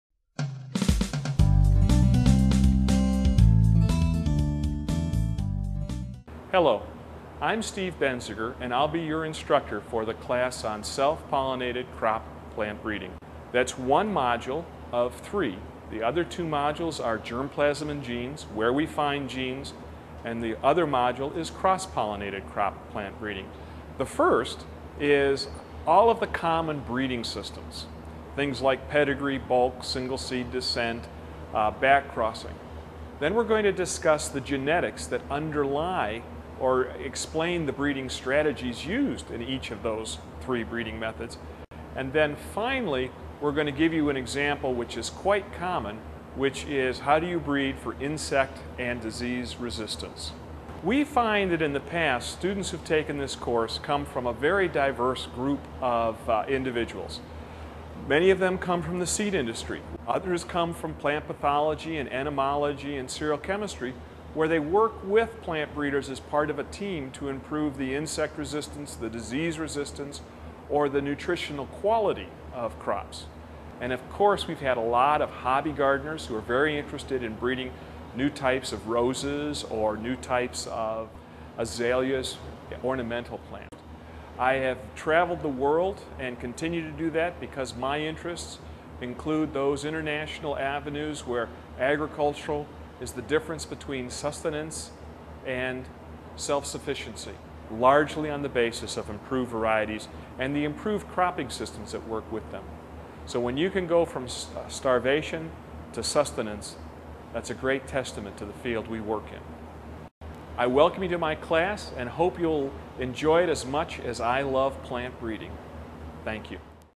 Self-Pollinated Crop Breeding Mini-Course Offered by UNL
First in a series of three distance-delivered crop breeding mini-courses offered by the Department of Agronomy and Horticulture at the University of Nebraska-Lincoln.  Courses are available for academic credit as well as noncredit professional development.